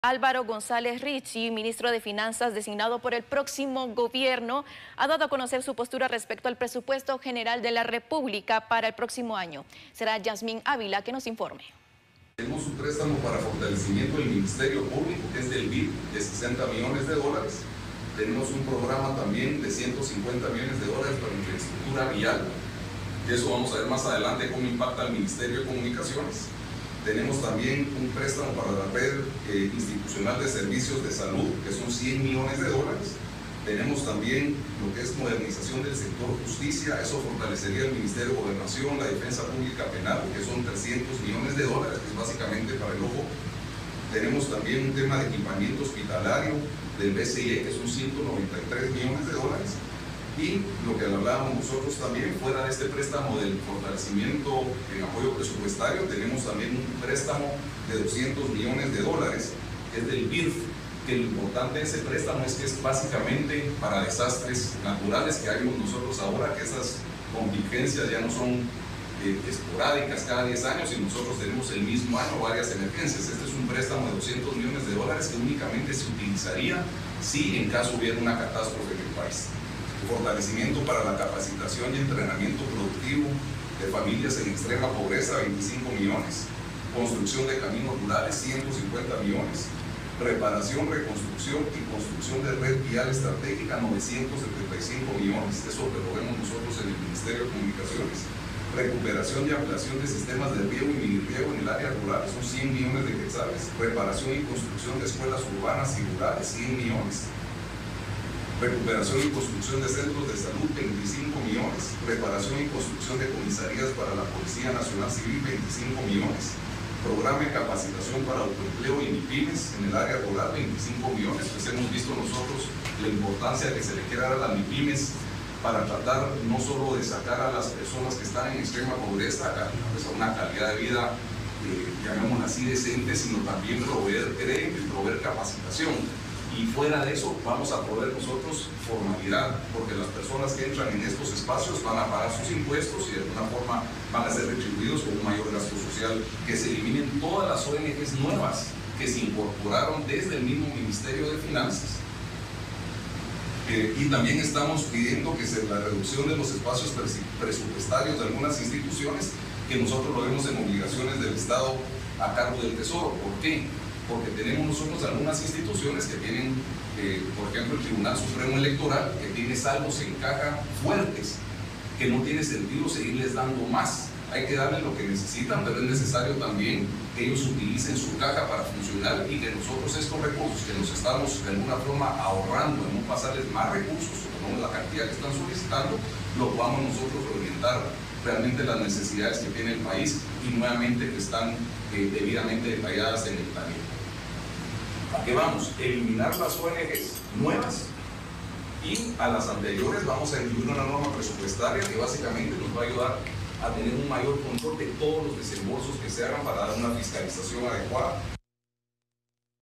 Próximo ministro de Finanzas opina sobre Presupuesto 2020
Álvaro González Ricci, ministro de Fianzas designado por el próximo gobierno, entregó su postura respecto al Presupuesto General de la República para el próximo año. González Ricci entregó sus conceptos sobre lineamientos en materia económica